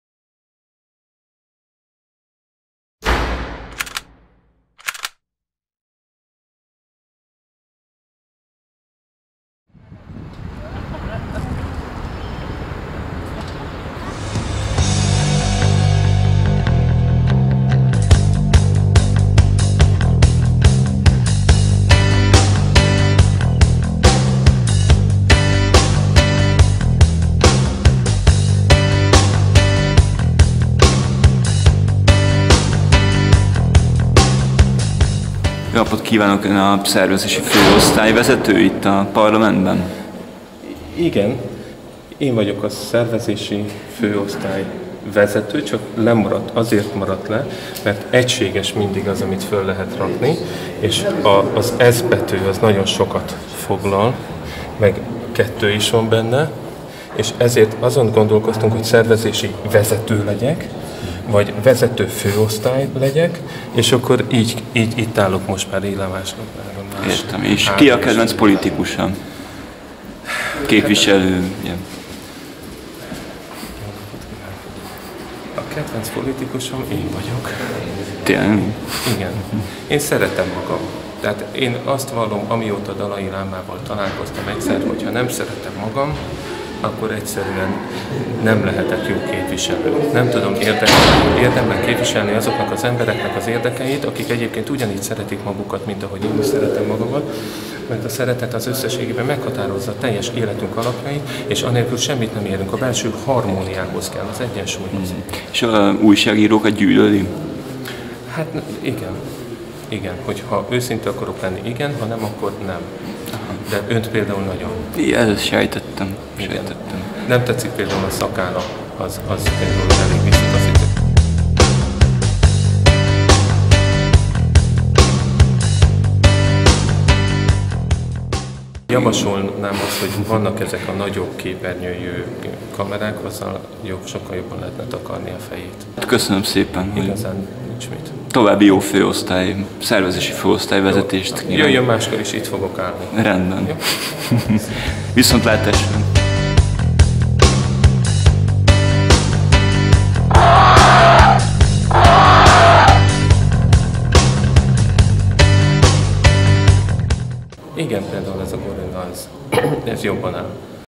A Politikus természetrajza 1. - Cifra Kaloda (HD)
"Én szeretem magam" - interjú egy igazán őszinte politikussal. Parlamenti portrésorozatunk első darabja.  Fércbók Blog Twitter